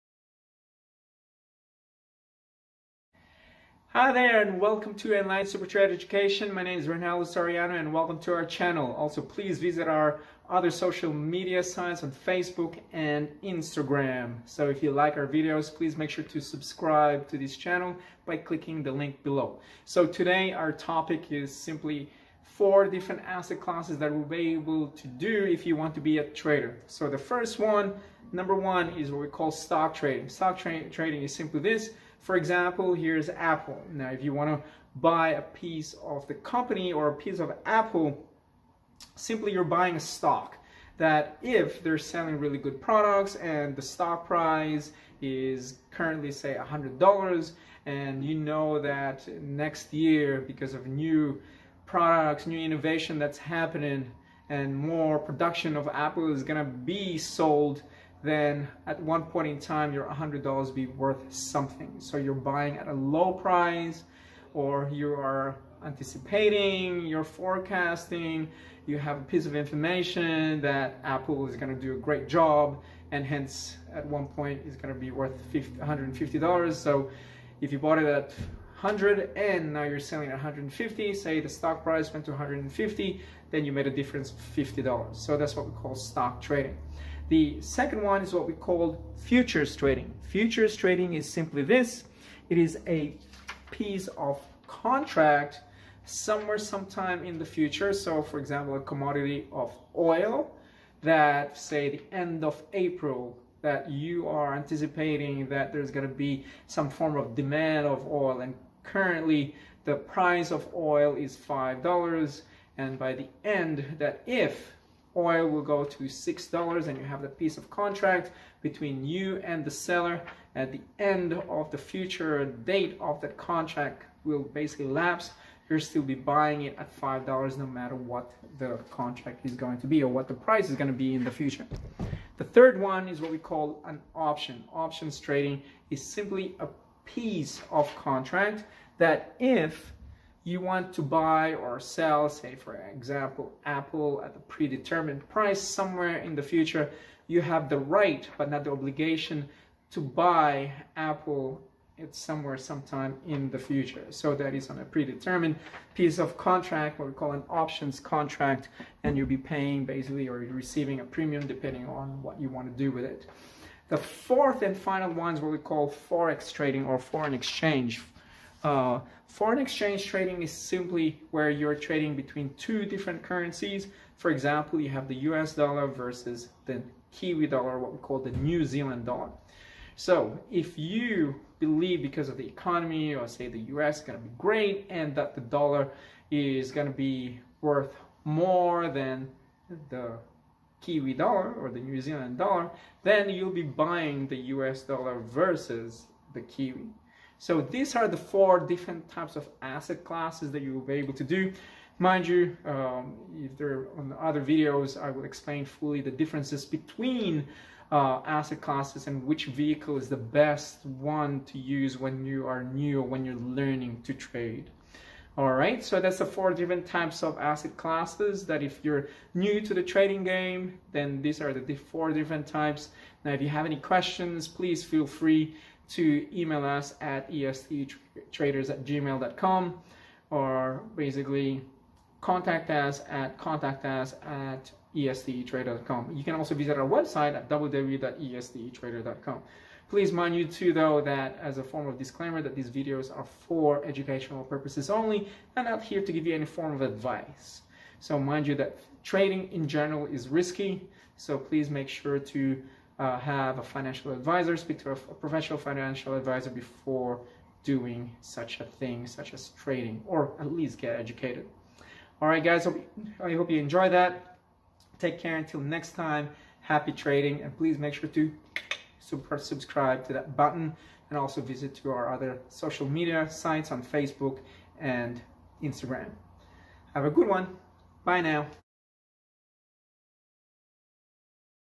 4 Different Asset Classes You Must Know If You Want To Be a Trader
Learn these 4 different types of asset classes if you want to be a trader.